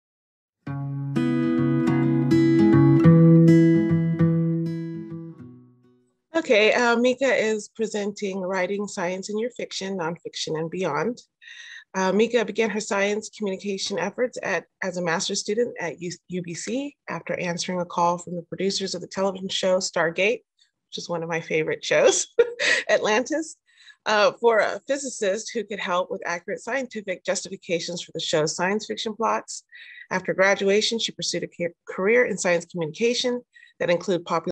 Science in Fiction with Mika McKinnon
Hosted by Cyn Bermudez
(edited by CynB)

Writing Science in your Fiction, Nonfiction, and Beyond

Have a science question? Is an earthquake shaking up the landscape in your fiction? Solar wind knocking out the satellites? Learn how to improve the science in your current work of fiction.
Applicable to all forms of fiction, genres, and nonfiction, from the murder mystery to the paranormal romance. Depicting more accurate science can elevate your story to the next level.

This workshop includes:
• Scientist for Fiction
• Writing science
• Utilizing your resources

Mika began her science communication efforts as a Master's student at UBC, after answering a call from the producers of the television show Stargate: Atlantis for a physicist who could help with accurate scientific justifications for the show's science fiction plots. After graduation, she pursued a career in science communication that included popular science writing and continuing her consulting role on the television shows Stargate: Atlantis and Stargate: Universe. More recently, McKinnon has consulted on Doomsday: 10 Ways the World Will End, No Tomorrow, Madam Secretary, and Star Trek: Discovery. In a profile with her alma mater, UCSB, she says that her interest in communication stemmed from the media's misrepresentation of a major landslide in the Pacific Northwest.

She was a contributing editor for Gawker Media, providing coverage on popular science topics for io9 and later became a science writer for Gizmodo. Her bylines cover topics including space exploration, dinosaur discovery, the convergence of science and art, and disaster preparedness. Her writing is in publications including Wired UK, Smithsonian magazine, Ars Technica, and Astronomy.
She volunteers for the National Academy of Sciences Science & Entertainment Exchange, providing subject matter expertise to the entertainment industry for more accurate depictions of science in the media.